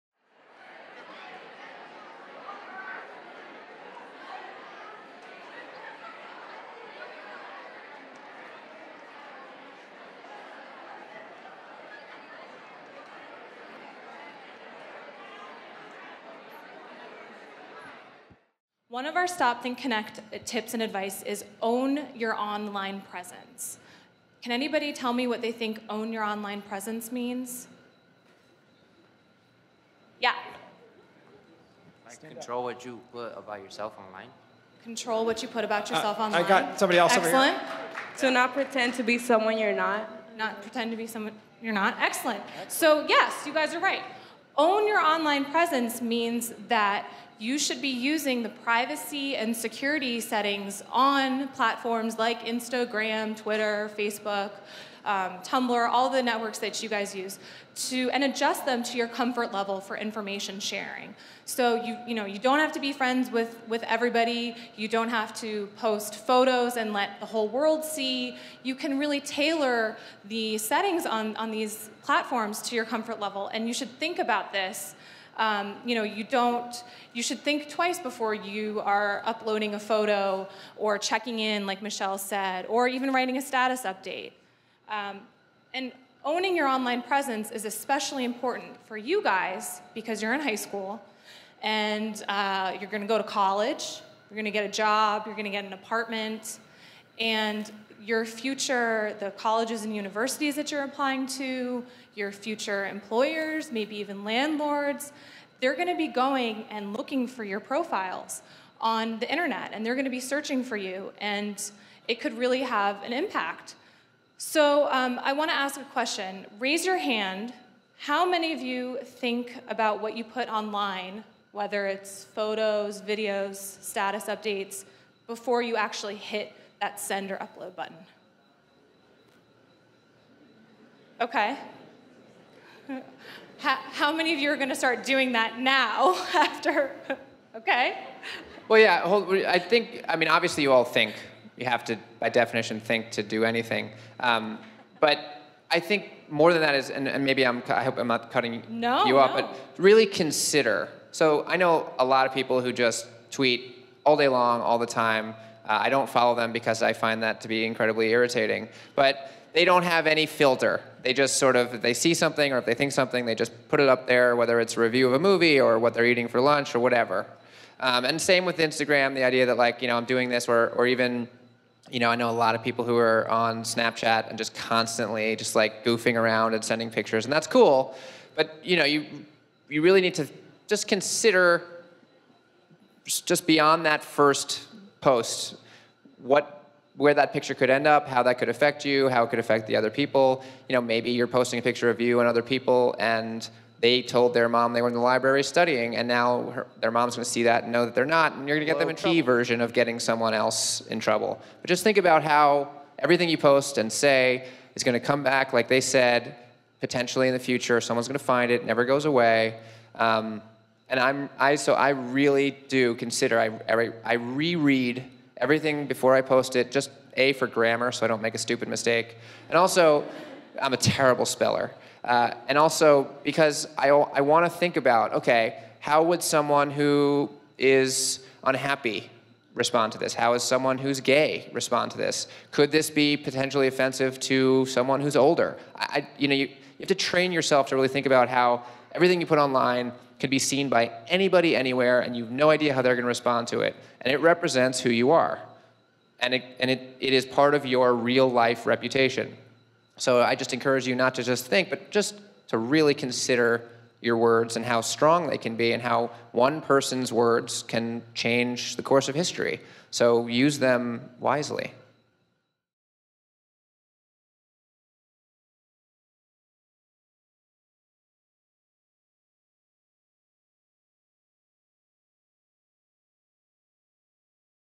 STOP. THINK. CONNECT. Tip: Own Your Online Presence
teamed up with Nev Schulman, host of MTV's "Catfish," and McAfee Vice President and Chief Privacy Officer Michelle Dennedy to talk to high school students at Gertz-Ressler High School in Los Angeles about online safety issues.